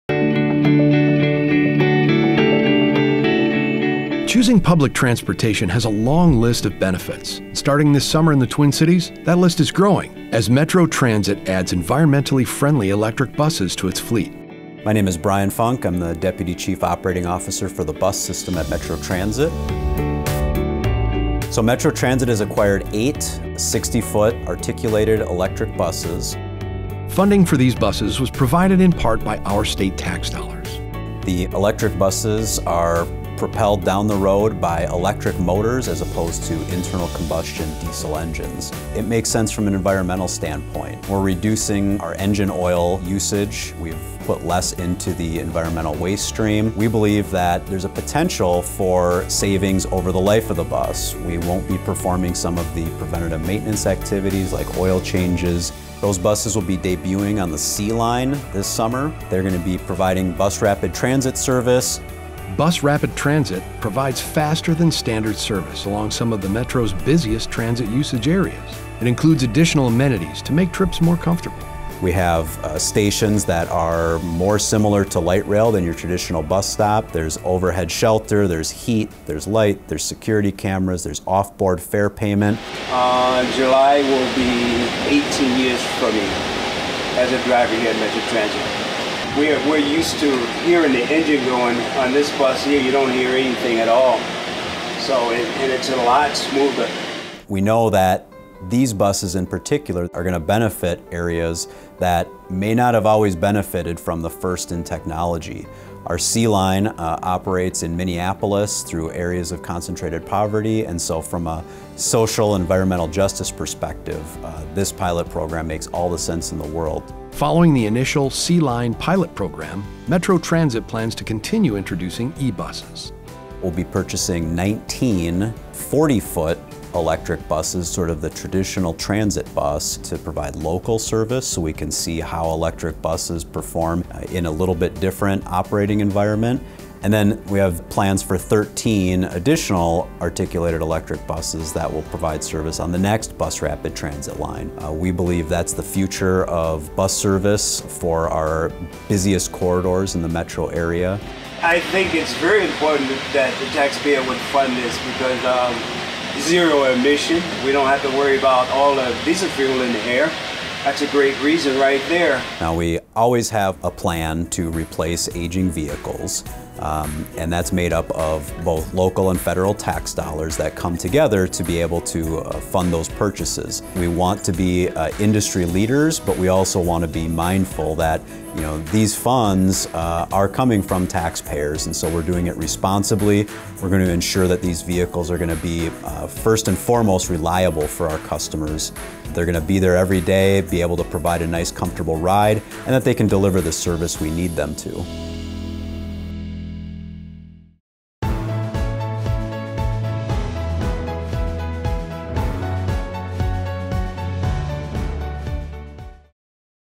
OurMN: Metro Transit Electric Buses
This summer in the Twin Cities, Metro Transit added environmentally-friendly electric buses to its fleet. These 60-foot articulated buses began providing Bus Rapid Transit Service on the C Line in June.

The C Line serves Penn Avenue from the Brooklyn Center Transit Center to Downtown Minneapolis. In addition to the reduced environmental impact of electric buses, the C Line provides faster service and additional amenities. Those amenities include off-board fare payment, security cameras, enhanced lighting, push-button heating, and more.

Metro Transit plans to continue introducing electric buses following their debut on the C Line. Funding for these buses is provided in part by our state tax dollars.